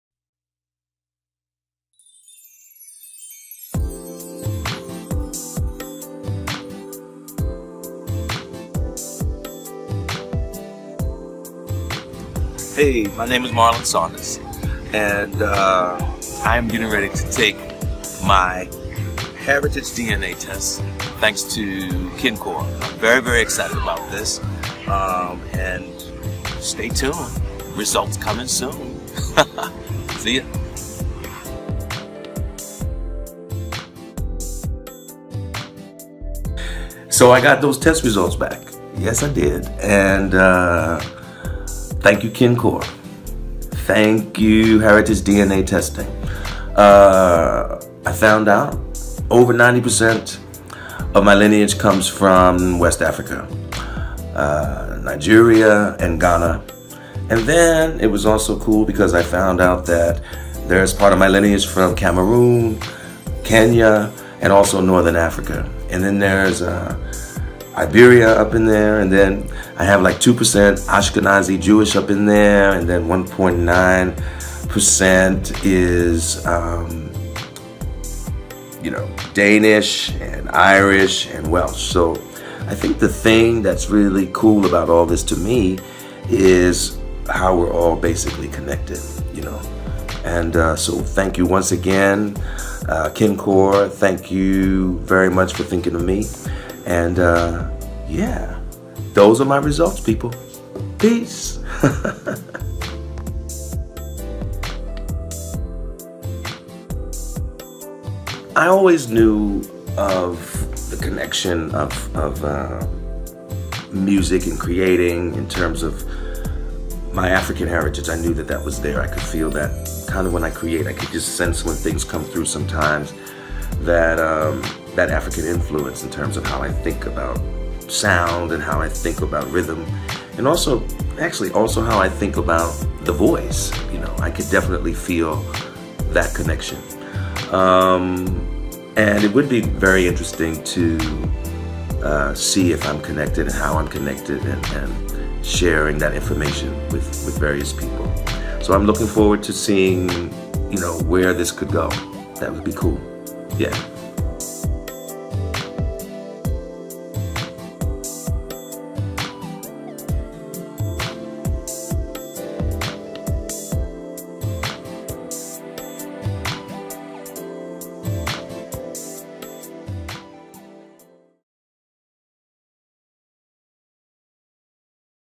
Marlon Saunders Ancestry DNA "Results" with KinCore.org
KinCore Influencer Marlon Saunders @MarlonSaunder shares his myHeritageDNA results with @KincoreOrg.